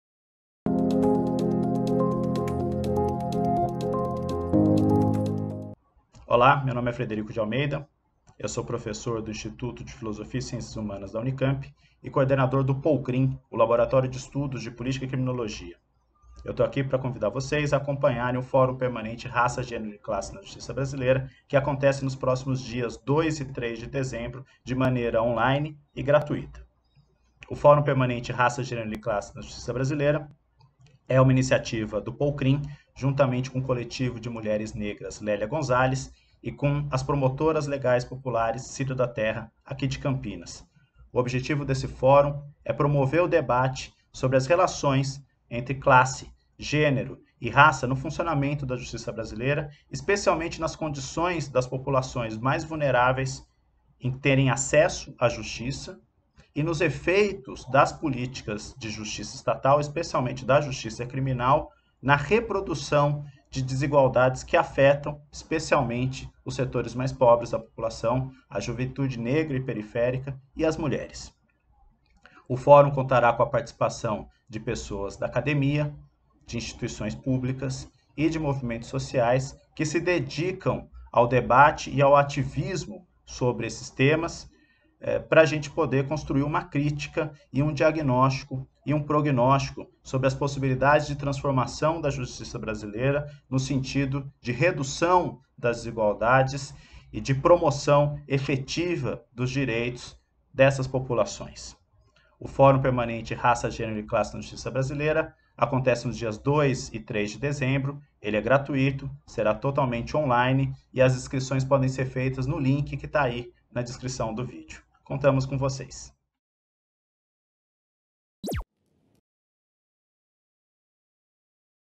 Fóruns Permanentes: Raça, gênero e classe na justiça brasileira
Fórum Permanente: Raça, gênero e classe na justiça brasileira
02/12/2020 a 03/12/2020

As edições de 2020 e de 2021 dos Fóruns Permanentes da Unicamp terão um novo desafio: a transmissão exclusivamente online. Os novos encontros serão transmitidos no site e nos canais dos fóruns no Youtube.